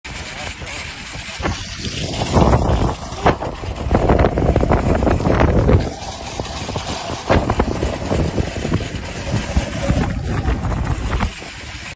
Felipe Drugovich’s Cold Adventures in Sweden🐕❄️
In this video Felipe Drugovich goes sleighing in Sweden with his friends.